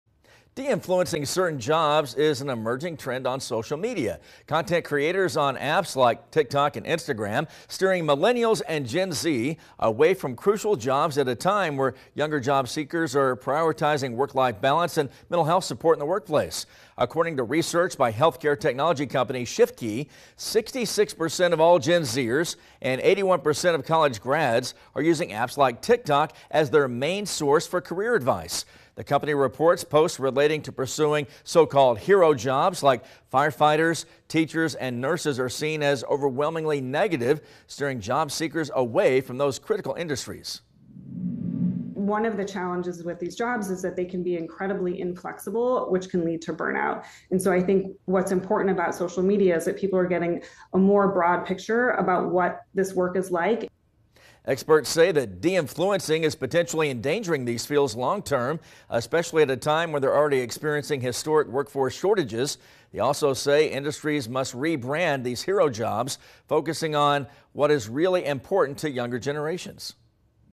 Social media deterring Gen Z from "Hero Jobs"
In a growing trend on apps like TikTok and Instagram, content creators are de-influencing certain jobs and steering Millennials and Gen Z from crucial jobs at a time when younger job seekers are prioritizing "work-life balance" and "mental health support".